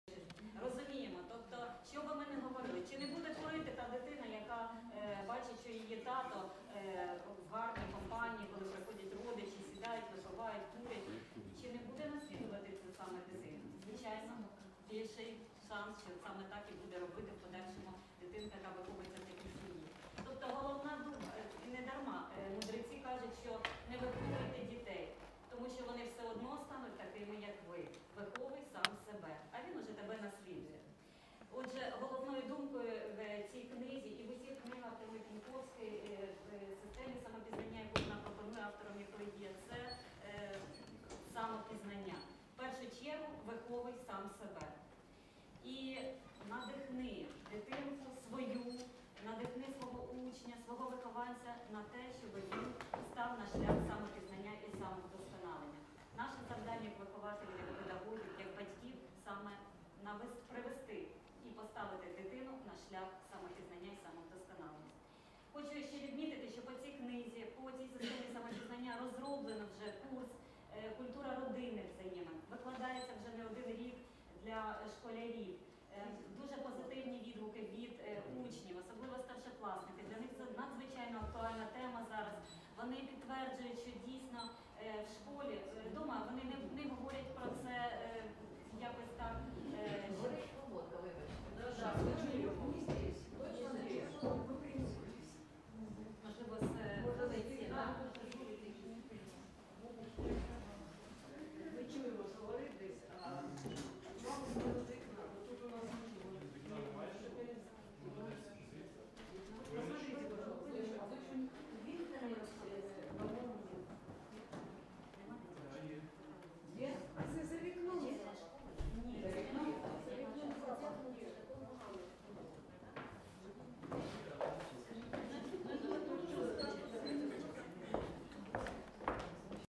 Эмма Пиньковская на мастер -классе с единомышленниками Киев 2017
Мы рады открытию Учителя счастья людей Эмму Пиньковскую. Слушаем ее единомышленникоу и присоединяемся.